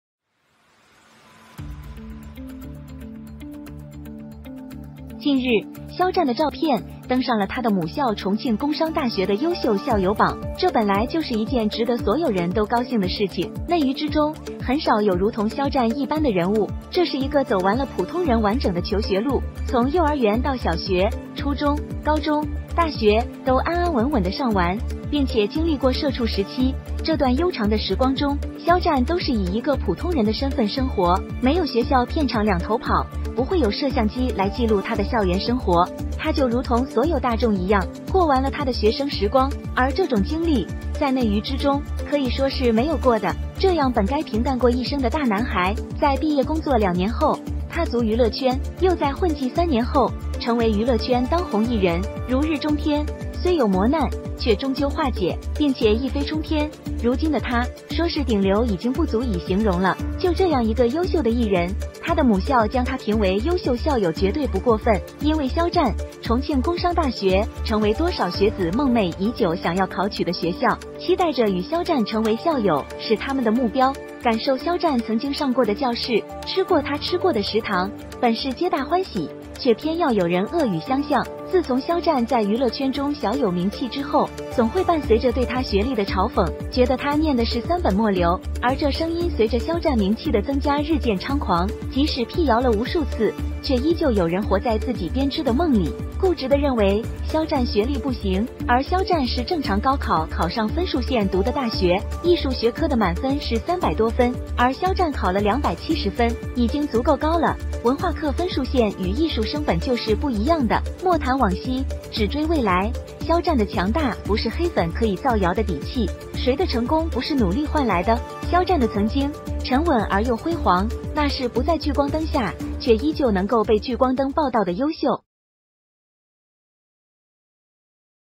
Xiao Zhan was ridiculed for being a well-known alumni of Gongshang University?
Recently, Xiao Zhan's photo was on the outstanding alumni list of his alma mater, Chongqing University of Commerce and Industry, which is something that everyone should be happy about! Among the inner entertainment, there are few characters as general as Xiao Zhan! This is a complete road to school for ordinary people, from kindergarten to elementary school, junior high school, high school, college are safely finished, and experienced the social animal period, this long period of time, Xiao Zhan is an ordinary person's identity life, no school film set both run, there will be no camera to record his campus life, he is just like all the public, finished his student life! And this experience, in the inner entertainment, can be said to have not lived! This should have been a bland life of the big boy, two years after graduating from work, set foot in the entertainment industry, and three years after the mix, become a popular entertainer in the entertainment industry, like the day, although there are trials and tribulations, but finally resolved, and soared to the sky, and now he, said the top stream is not enough to describe! His business value and his own strength, is definitely the top existence! Such an excellent entertainer, his alma mater will be his excellent alumni is absolutely not too much! Because of Xiao Zhaoge, Chongqing University of Commerce and Industry has become a school that many students have dreamed of for a long time, looking forward to becoming alumni with Xiao Zhaoge is their goal, feel Xiao Zhaoge once on the
The classroom, ate his canteen! Chongqing University of Commerce and Industry has become famous because of Xiao Zhan is definitely not just talk, because a person in love with a city examples abound, not to mention his alma mater? It was all good, but someone had to speak ill of it! Since Xiao Zhaoge became famous in the entertainment industry, it was always accompanied by mockery of his education, thinking that he had studied at the end of the third grade, and this voice became more and more rampant as Xiao Zhaoge's fame increased! Even after countless rumors, there are still people who live in their own woven dreams and stubbornly believe that Xiao Zhan's education is not good! And Xiao Zhan is the normal college entrance examination score line to read the university, the full score of art subjects is more than three hundred points, and Xiao Zhan took two hundred and seventy points, is high enough, the cultural course score line and art students is not the same! The university that Xiao Zhan went to is the real Chongqing University of Commerce and Industry, Xiao Zhan is in the Orchid Lake campus and the main campus status is the same, and not an independent institution! And during school, in 2012, Xiao was responsible for the design of NED, completed all the works, and finally won the public welfare theme poster design award! Even after joining the X Nine Youth Group themselves, they were able to exhibit the posters they had remitted from their college time and not fall into the cliché! It can be said that both Xiao Zhan's education and Xiao Zhan's ability, and even Xiao Zhan's social influence today, are fully qualified to enter Chongqing IIT's alumni list! And not as the black powder rumors, just a three only! The fact that Xiao can walk through that 2020 storm is proof enough of how strong his heart is, can survive, but does not mean there is no impact, when hearing his own admonition is "Xiao, you have to live well ah", whether he also wanted to leave it? Do not talk about the past, only chase the future, Xiao Zhan's powerful is not the bottom of the black powder can be rumors, who's success is not hard work in exchange? Xiao Zhan's once, calm and brilliant, that is not in the spotlight but still able to be spotlighted to report the excellent!